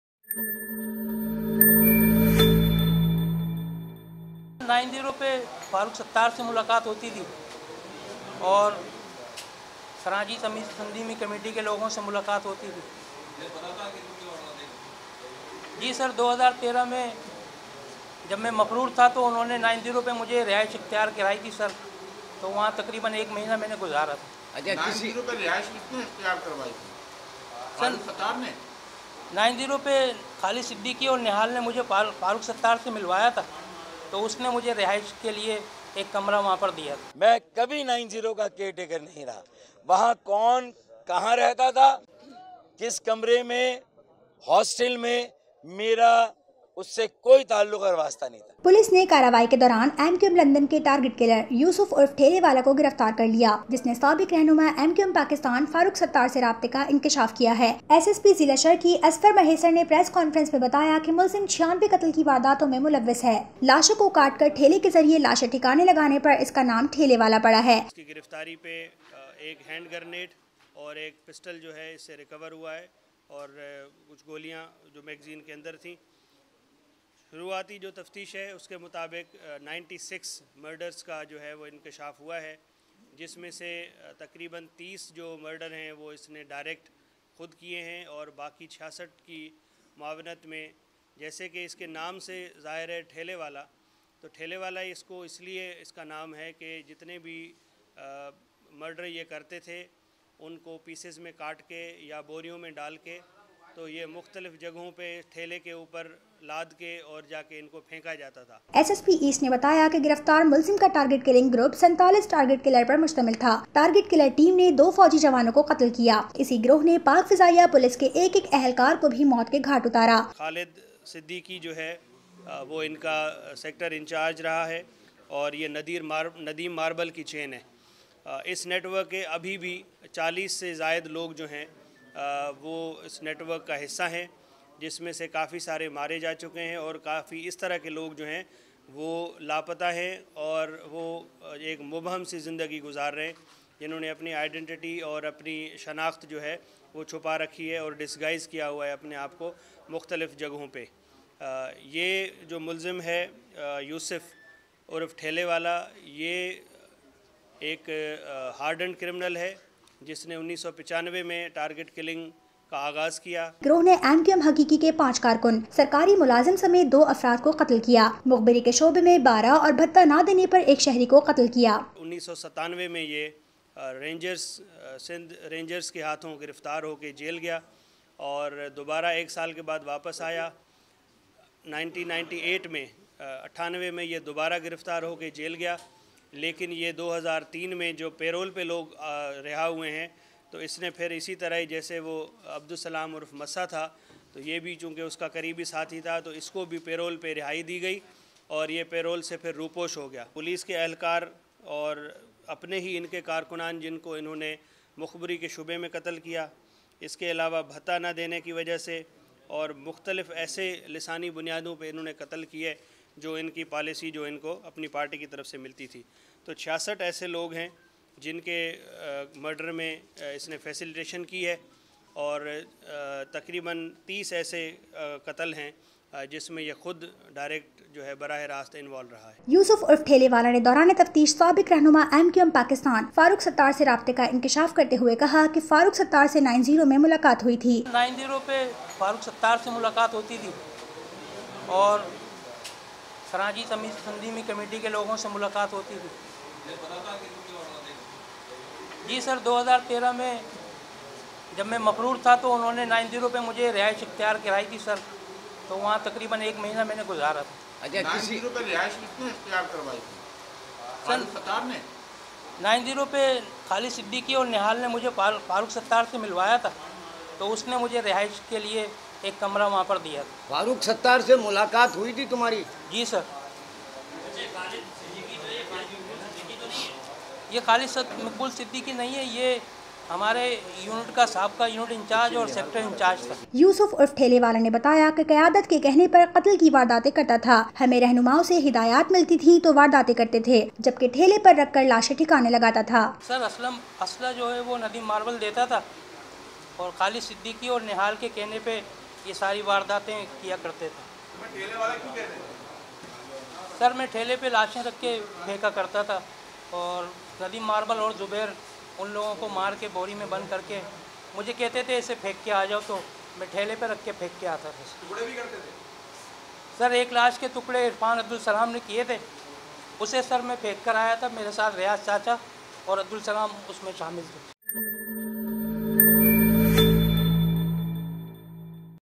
MQM London ka Karinda Giraftar 96 Qatal Qabool kar liye
~Yousaf @Thelewala Griftar Mulzim ka Bayan ; 90 pr Farooq Sattar se Mulakat hoti thi, 2013 se mei farar tha toh 90 pr Rehaish  di 1 month .
Gang mein 37 Mulzeman  aur hain jo Target killing karte hain . Khalid Sidiqui aur Nadeem Marbal ki chain ka hissa hain .
~Killings: 2 Militant, Police Officers , and Civilian People.
~Total Killings 96
~TT Pistol with Some Rounds, HandGrenade